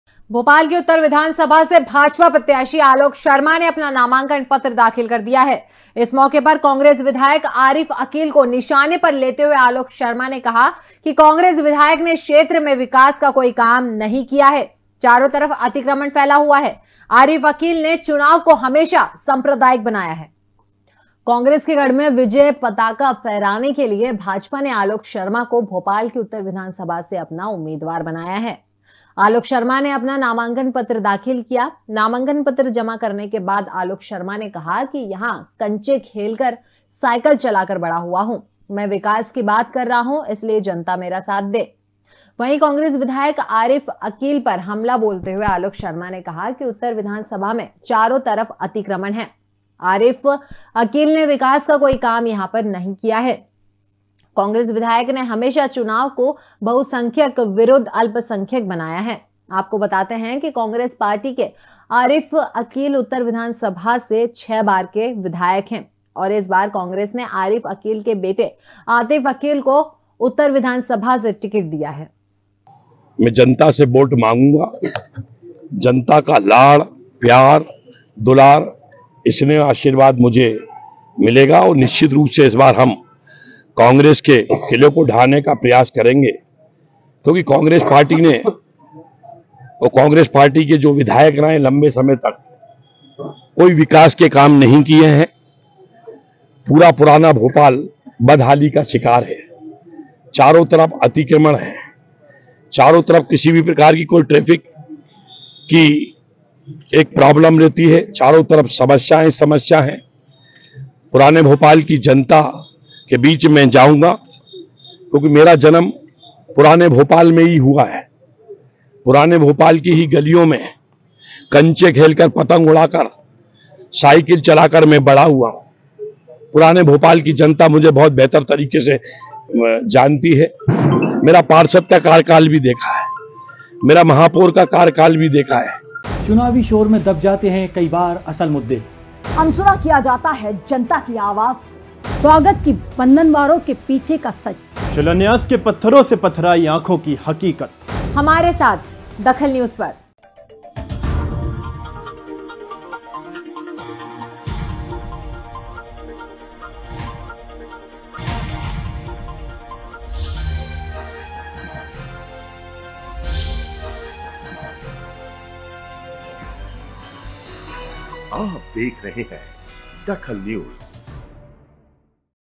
DAKHAL NEWS आलोक शर्मा ने नामांकन दाखिल किया
आलोक शर्मा ने नामांकन दाखिल किया
आरिफ अकील पर साधा तगड़ा निशाना